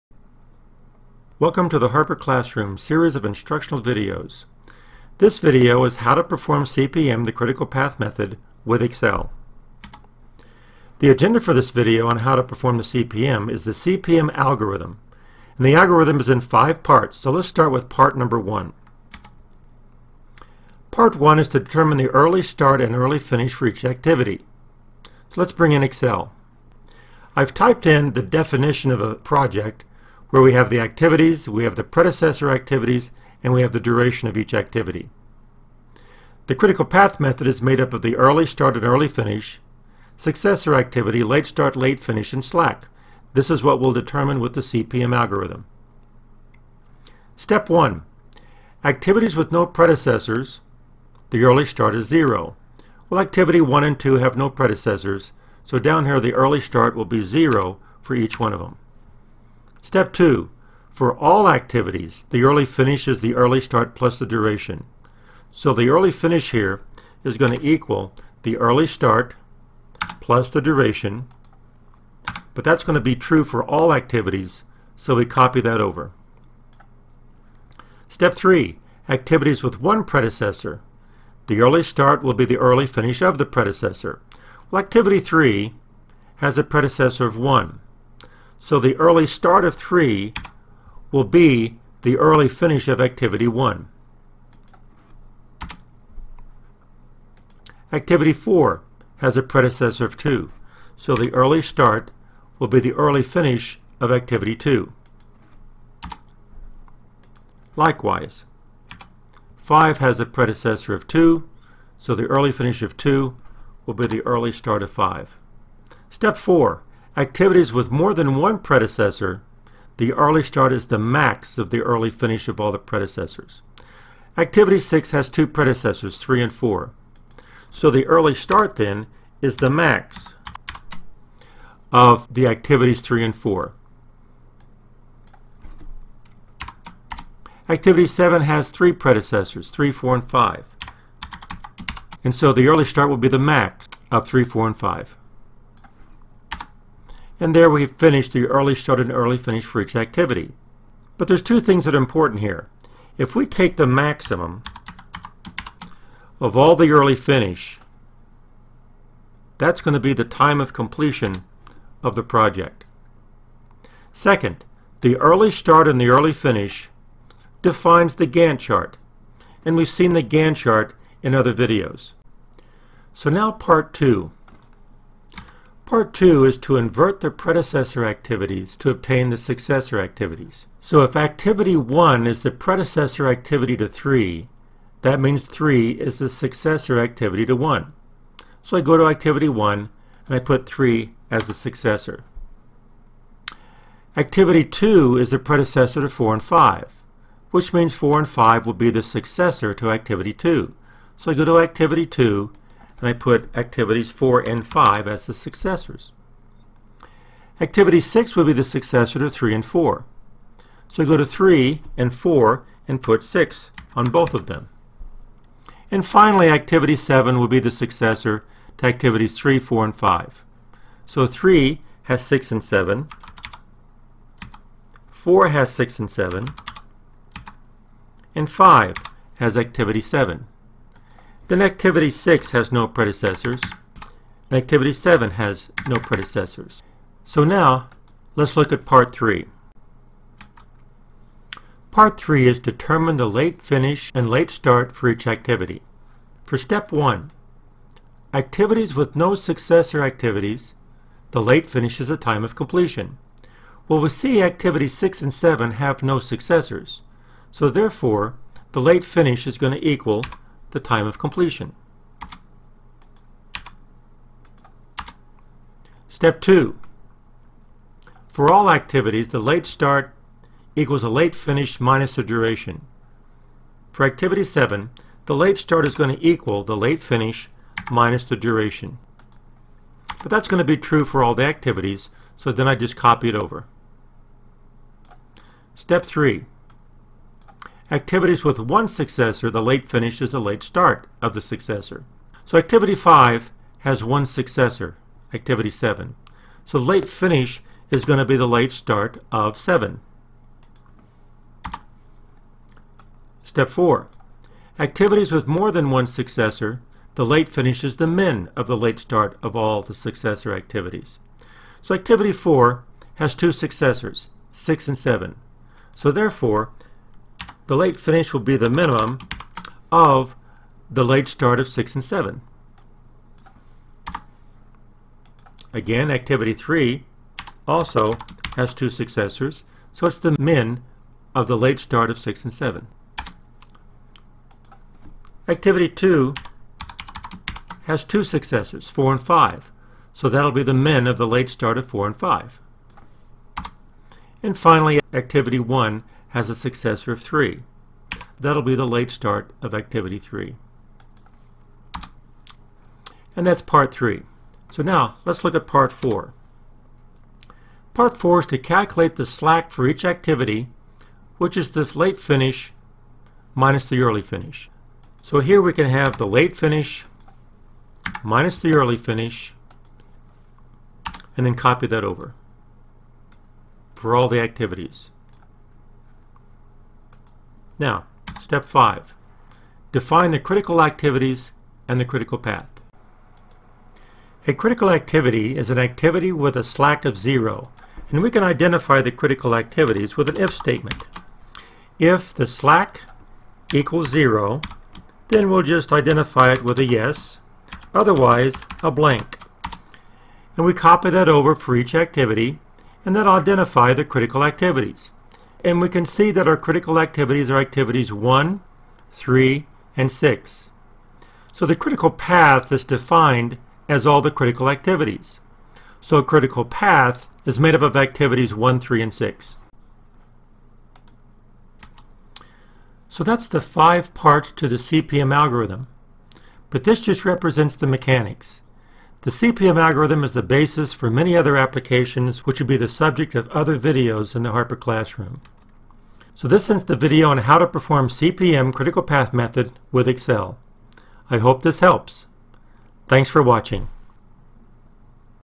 Critical Path Method-CPM-with Excel | Dr. Harper's Classroom
This HOW TO video will teach the mechanics of performing the Critical Path Method (CPM) using an algorithm with Excel.  The CPM algorithm will produce early start, early finish, late start, late finish, and slack.  The critical path, the time of completion, and the Gantt chart in table form are also defined.  Videos in the Harper Classroom are conveniently organized by content at the website "MDHarper.com".